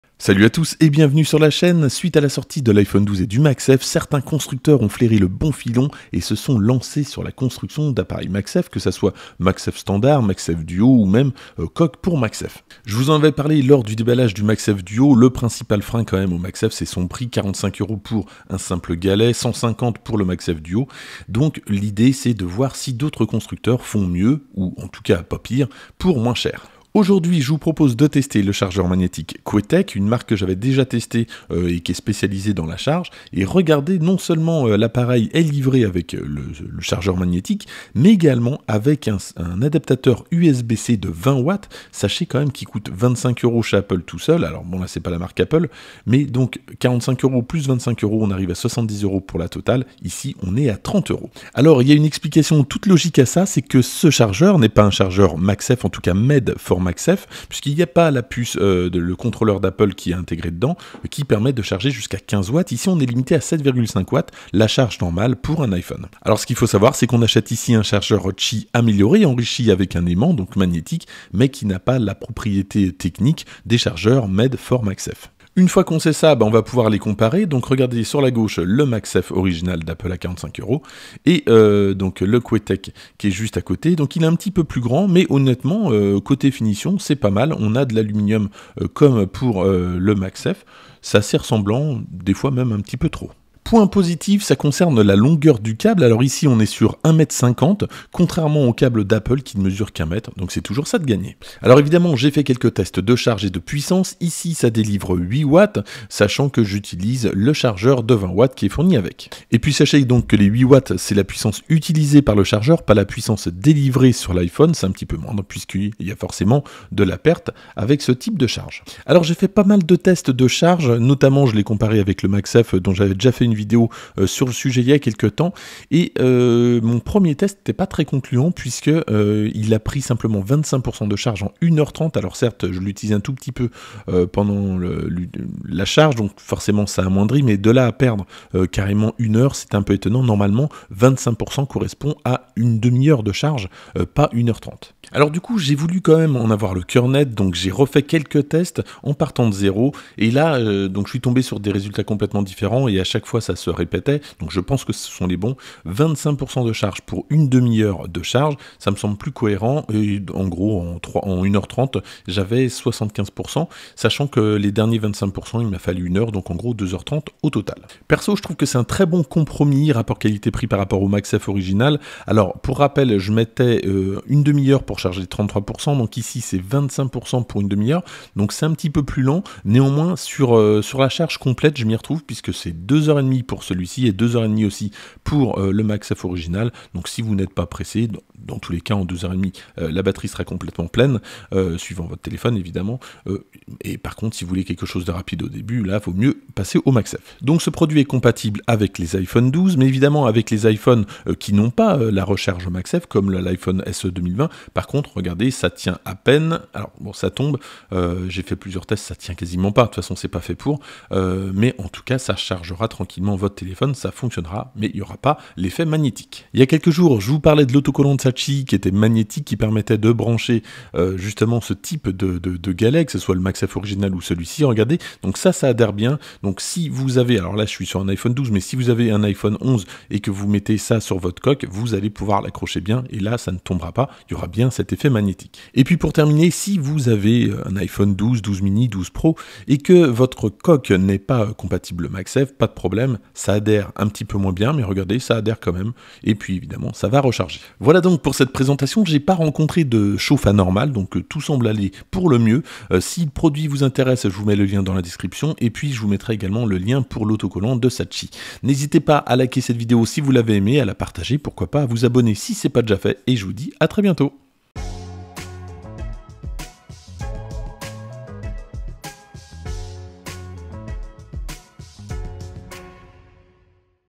Chargeur magnétique CHOETECH : Un concurrent pas cher au MagSafe
Je teste un chargeur à la Magsafe de la marque CHOETECH. Ce chargeur 7,5w pour iPhone est non seulement moins cher mais également fourni avec un adaptateur secteur USB-C PD de 20w !
Lien sur Amazon du chargeur CHOETECH